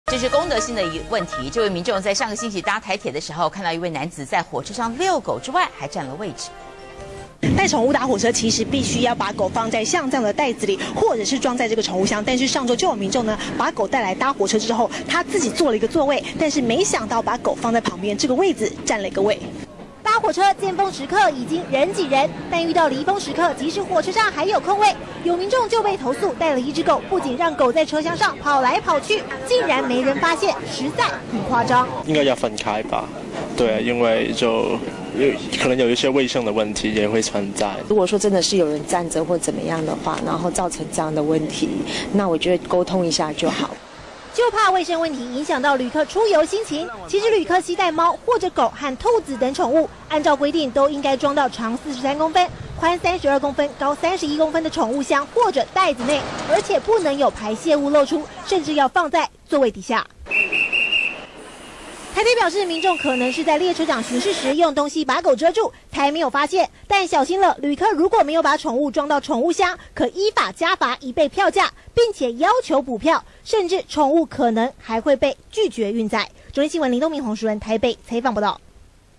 扯！男子帶狗搭火車又占位 放任車廂奔跑
中天新聞》有民眾投訴，一名男子帶狗搭乘火車，不但沒有依法將狗放在寵物箱，還放任狗在車廂內奔跑，更誇張的是讓狗坐在旁邊的位置，占了一個座位，因此，拍照下來投訴台鐵，台鐵表示，若民眾帶貓狗搭乘火車，沒有依照規定將貓狗放入寵物箱中，可能會被罰款或者拒載。